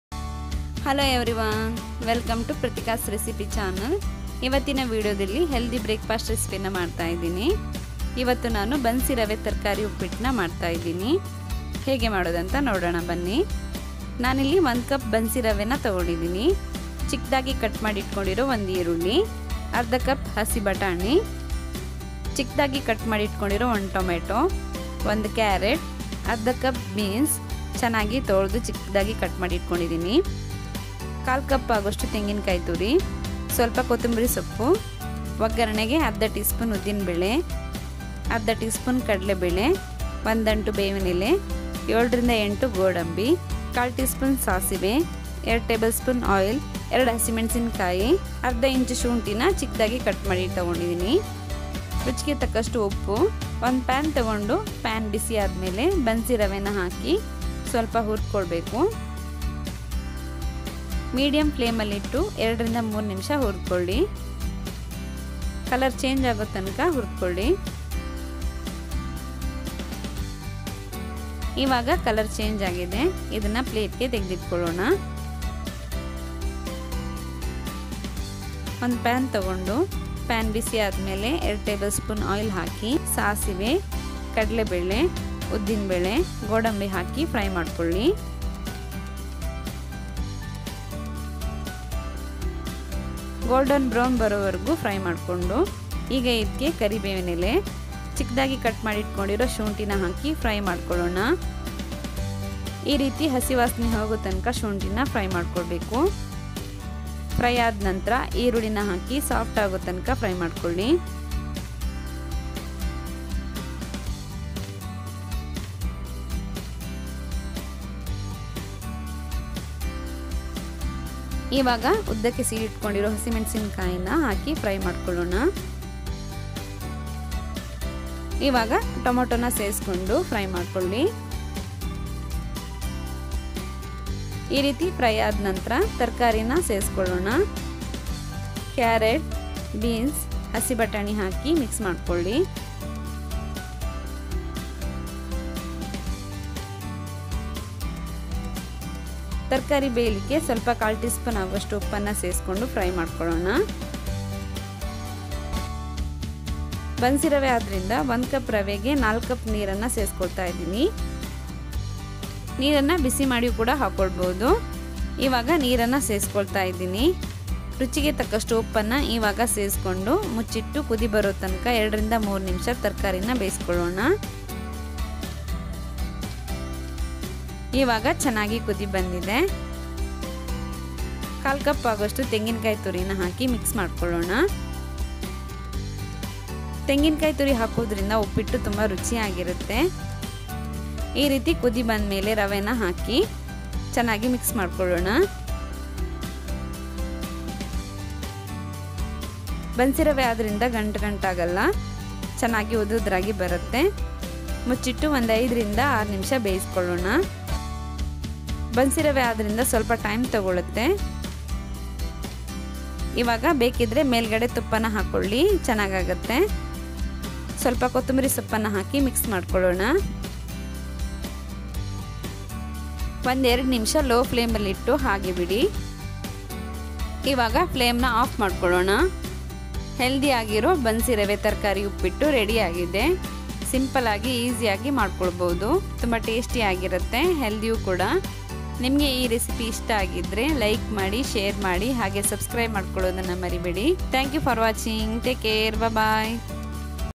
ಬನ್ಸಿ ರವೆ ತರಕಾರಿ ಉಪ್ಪಿಟ್ಟು /Bansi Rava Vegetables Upma/Bansi Rava Uppittu/Veg Upma Recipe in Kannada
Vegetables Upma Recipe/Bansi Rava Vegetable Upma/Bansi Rava Uppittu/Upma/Veg Upma Recipe in Kannada
lowcaloriebreakfastrecipe vegetables bansiravaupmarecipe

Ingredients:
Bansi Rava - 1 cup
Onion - 1
Green Peas - 1/2 cup
Tomato - 1
Carrot -  1
Fresh grated Coconut -  1/4 cup
Urad dal - 1/2 tsp
Chana dal - 1/2 tsp
Curry leaves
Cashewnut - 7 to 8

@Prathika's Recipes ಕನ್ನಡ